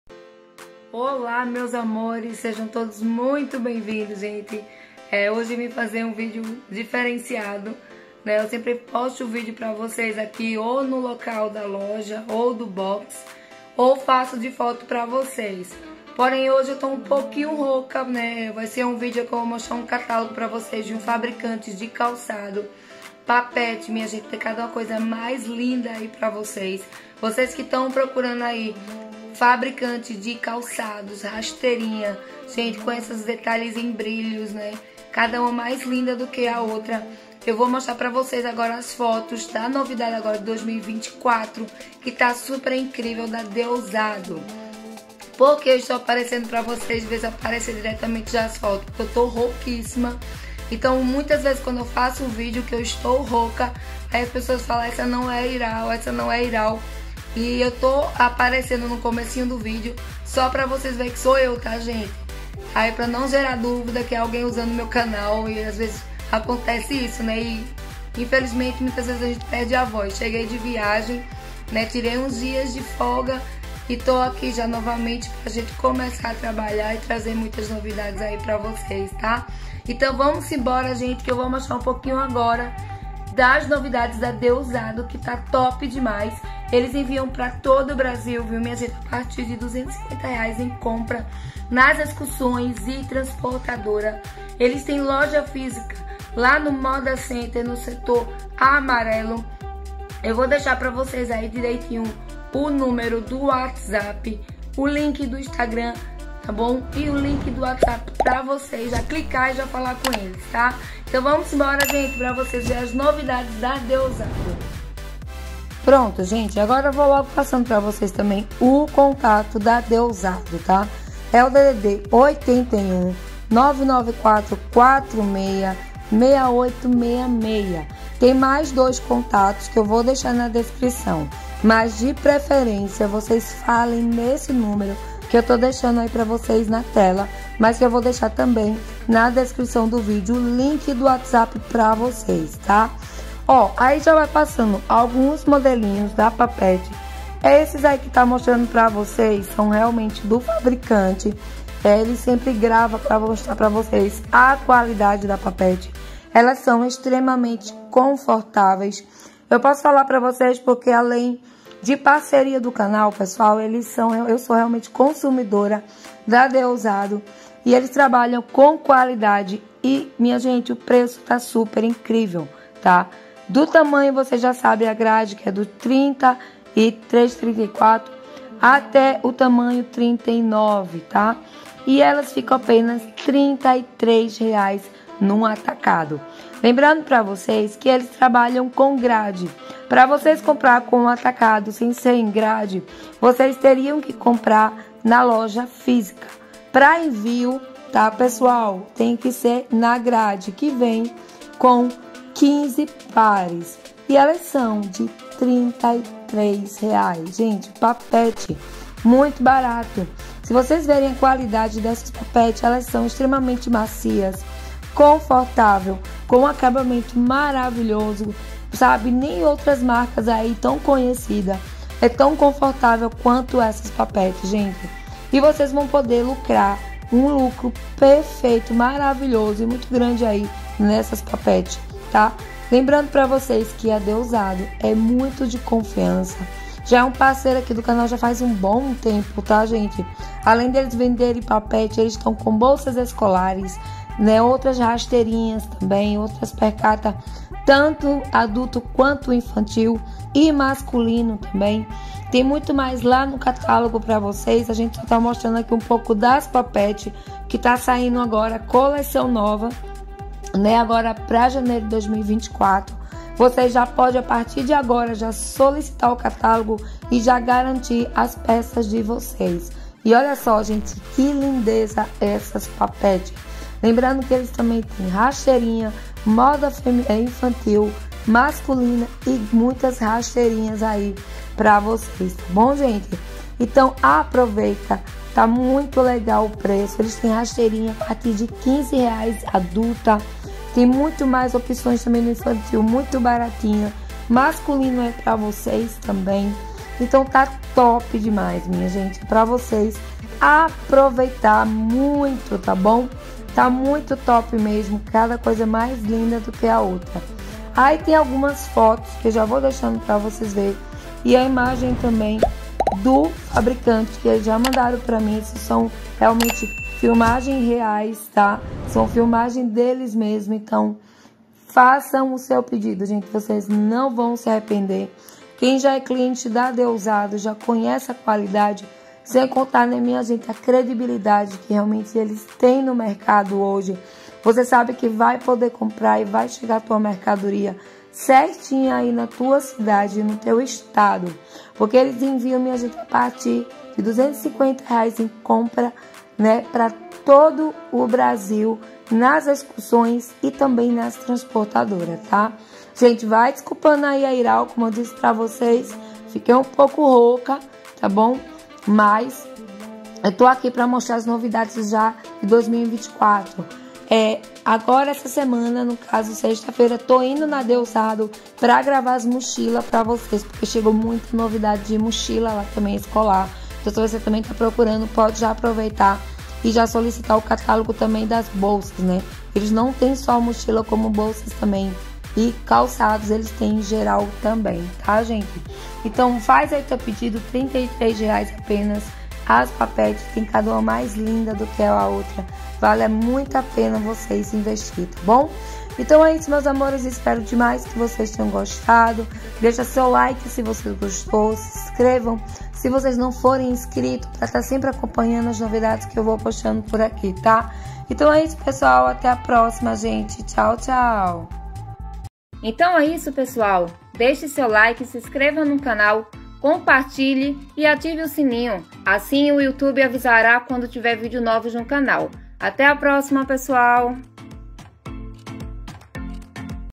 FÁBRICA DE CALÇADOS DE SANTA CRUZ DO CAPIBARIBE PE
Olá amores, sejam bem vindos. Pra quem não me conhece me chamo Iral e pesquisei bolsas, carteiras, cintos e calçados. Espero que gostem.

Fale com o Fornecedor The Ousado Diretamente pelo Link do Whatsap